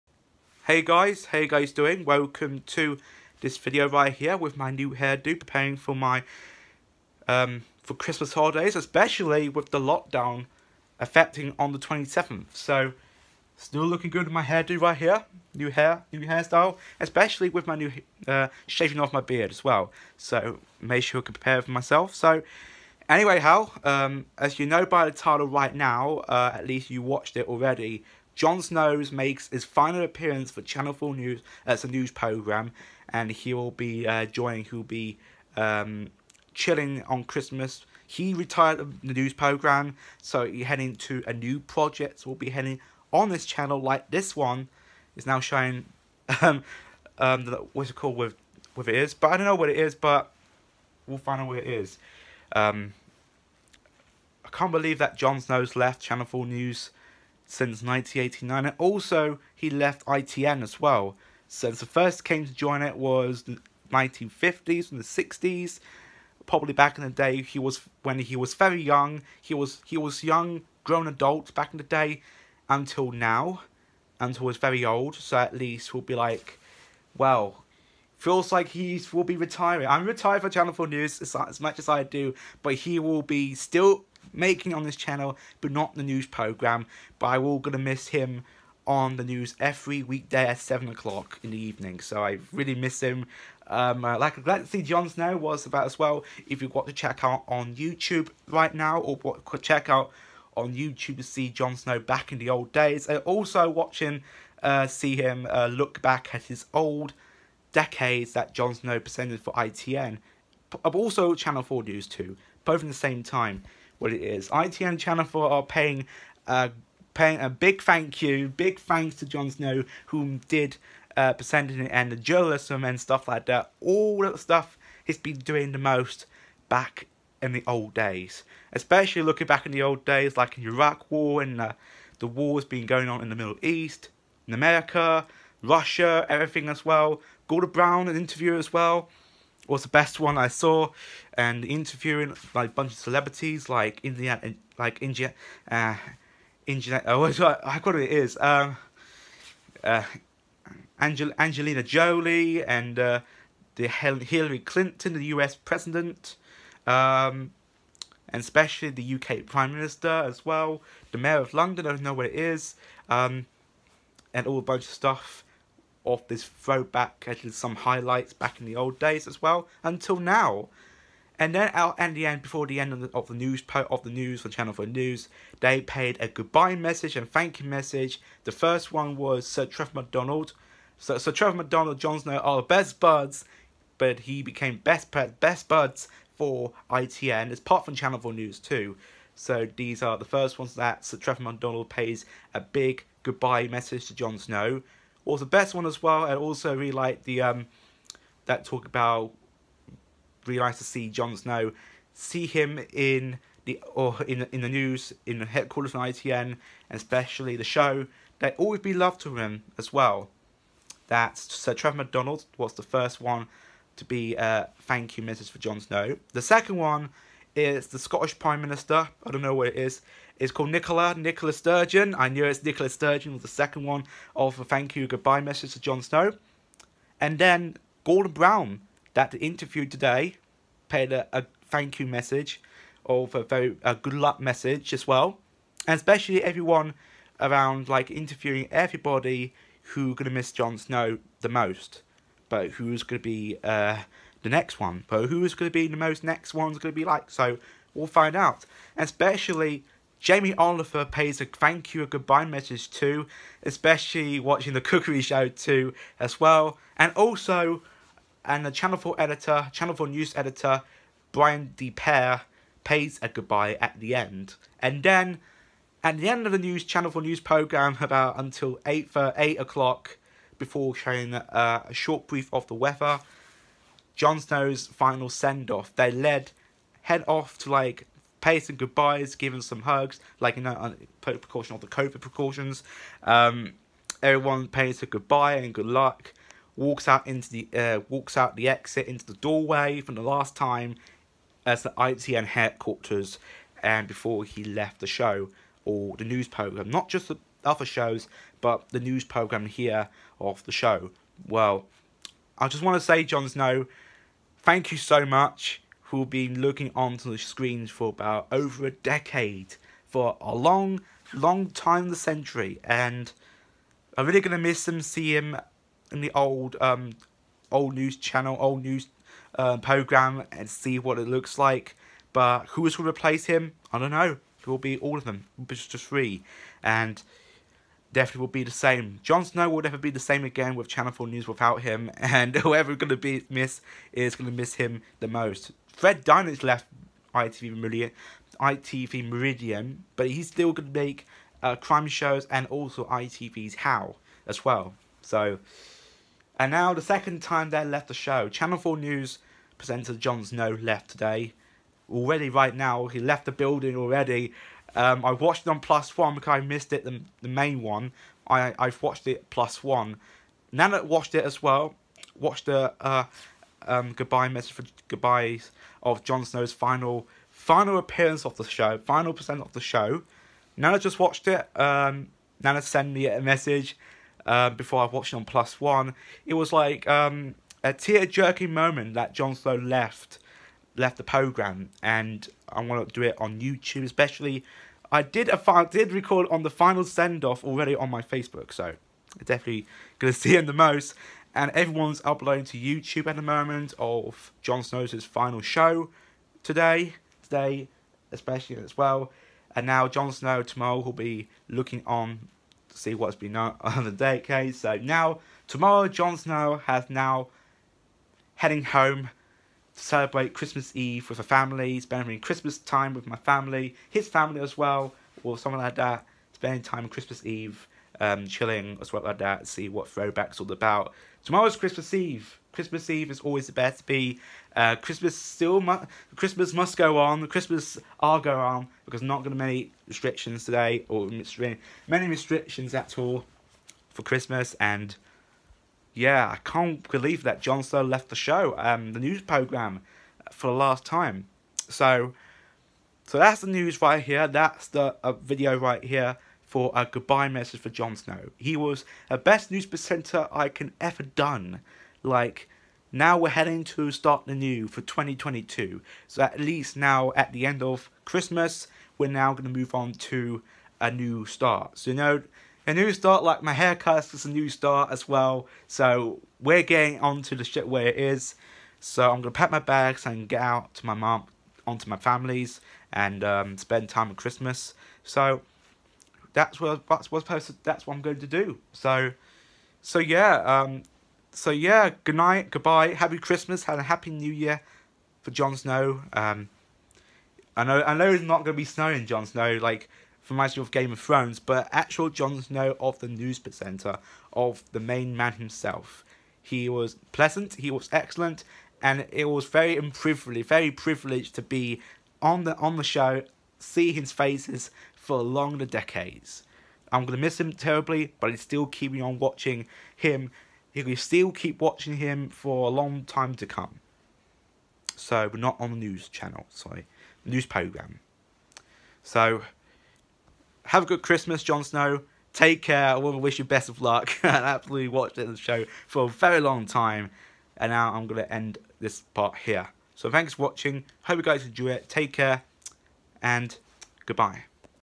Jon Snow is leaving Channel 4 News
Here’s a message to Jon Snow because it’s his last day today of Channel 4 News.

I wanna wish him goodbye and have a Happy Christmas or should I say “That’s Channel 4 News good evening”.